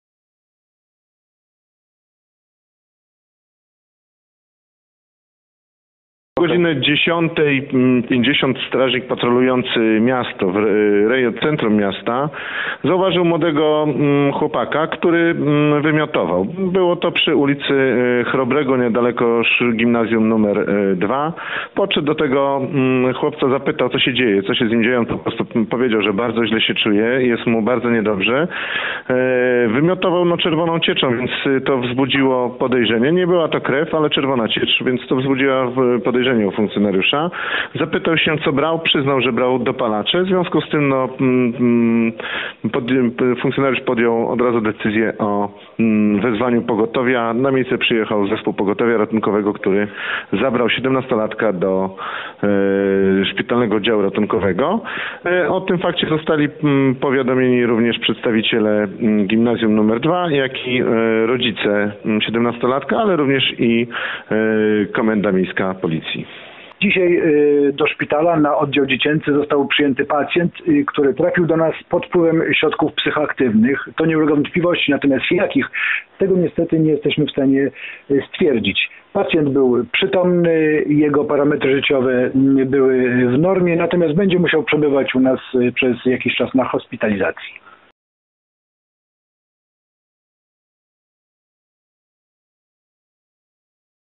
17-latek po dopalaczach w szpitalu
Chłopak mocno wymiotował czerwona cieczą, nie była krew - mówi Piotr Simiński, komendant Straży Miejskiej w Koszalinie. 17-latek trafił do koszalińskiego szpitala.